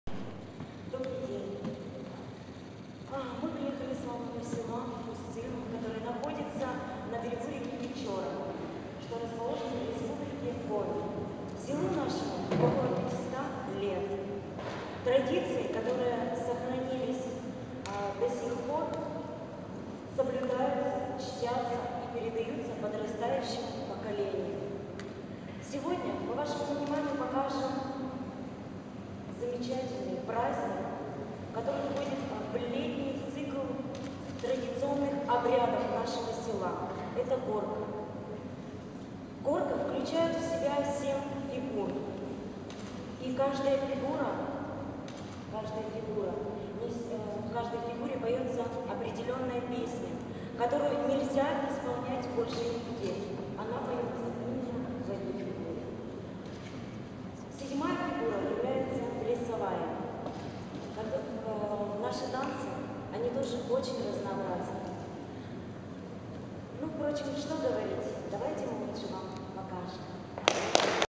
Горка (вступление) с.Усть-Цильма (Республика Коми)
30, 31 октября 2013 года.
Международный Фольклорный Фестиваль "ПОКОЛЕНИЯ"
Арт-проект Ольги Леман-Балашовой,
посвященный 10-ти летию ОАО "Российские Железные Дороги"
В программе:
Оркестр "ЦАРСКАЯ БАШНЯ"
художественный руководитель Ольга Леман-Балашова;
Фольклорный ансамбль села УСТЬ-ЦИЛЬМА (Республика Коми);
ДОМЕНИКА НИКИФОРОВНА ЧЕКУН (Украина);
Фольклорный ансамбль села КРУГЛОЕ (Красненский район, Белгородской области);
КУТОЧАНЕ (Украина).